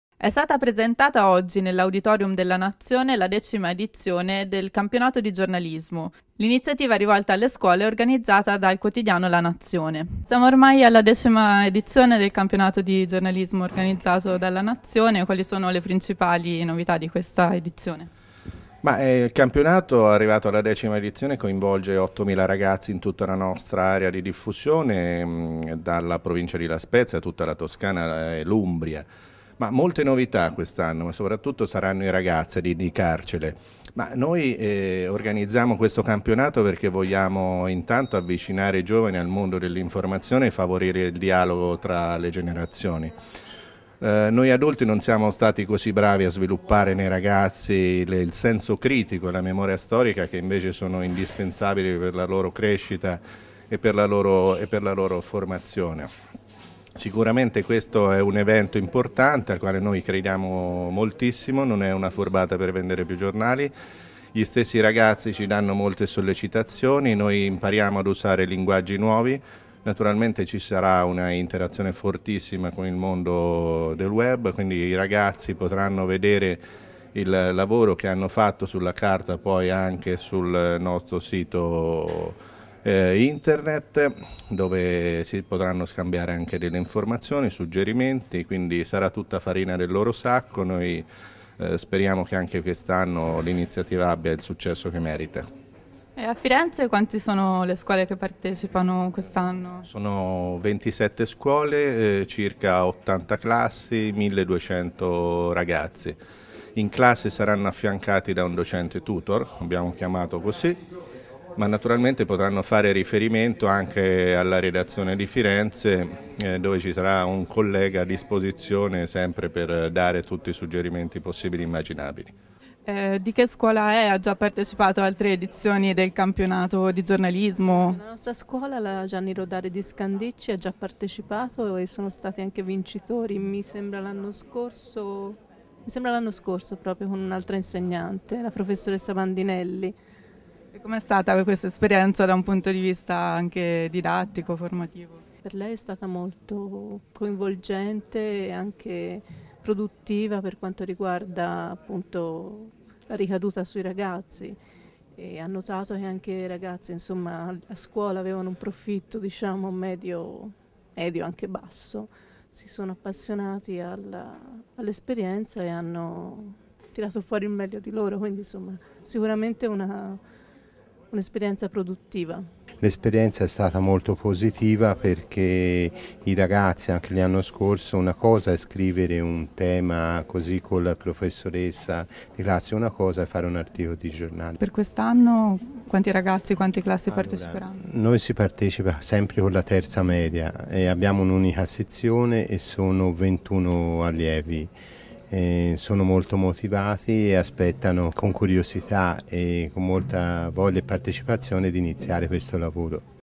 Presentata la decima edizione del Campionato di giornalismo
Servizio di Guia Baggi